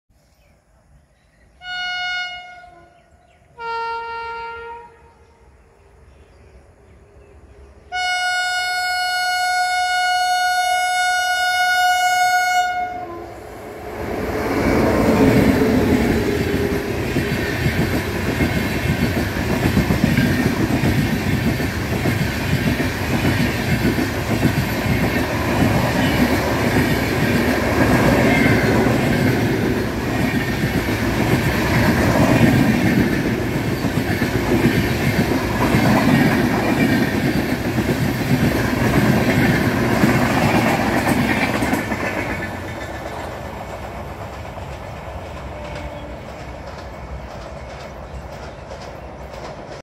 Strong and Steady || Furious Honking WAP7 with Kamrup Express takes through Level Crossing
HWH WAP7 Furiously honks with 15959 Howrah - Dibrugarh Kamrup Express on its scheduled time crosses Thakurnagar Level Crossing near New Jalpaiguri.

Videograhy by - Indian Railway Fantasy